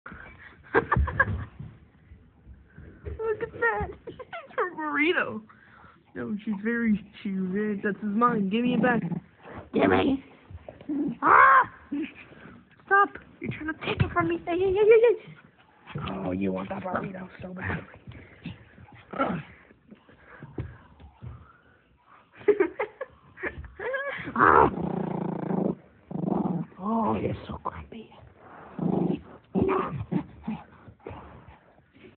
My dog took something of mine
It was very squishy she prob wanted to pop it